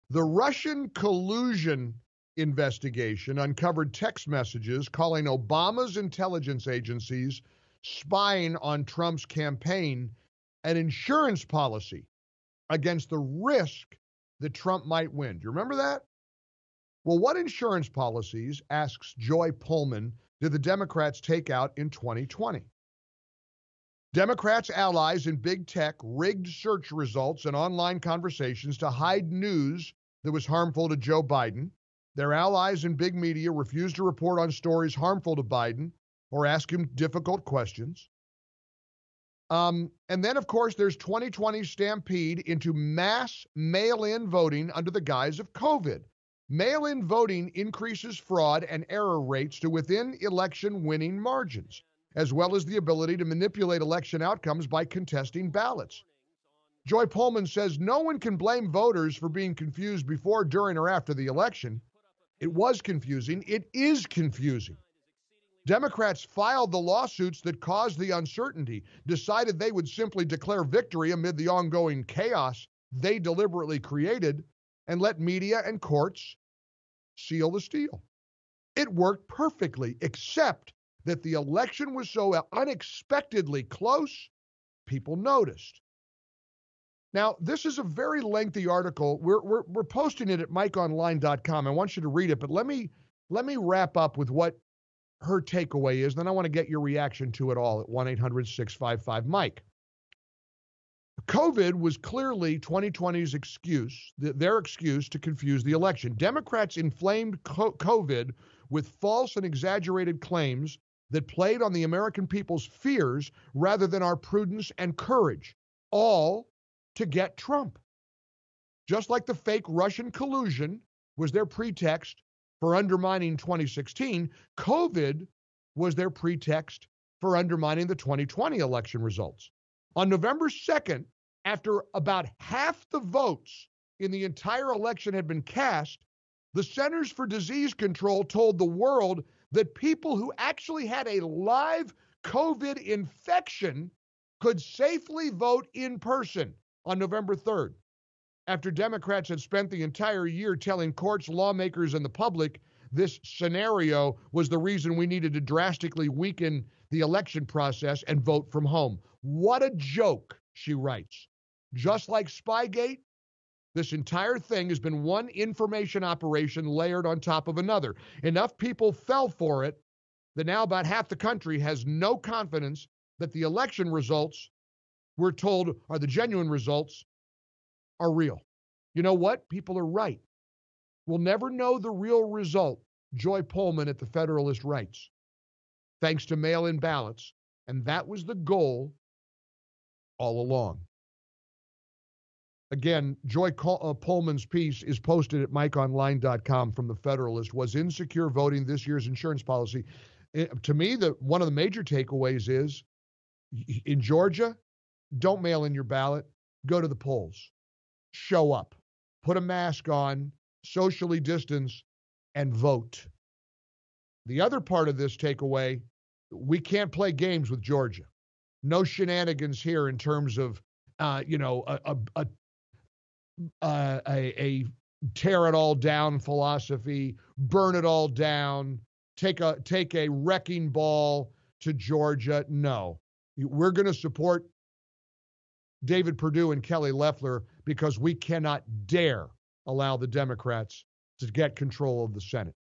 Was insecure voting this year’s ‘insurance policy’ for Democrats
Mike reads an article from The Federalist called “Was Insecure Voting This Year's 'Insurance Policy' For Democrats?”.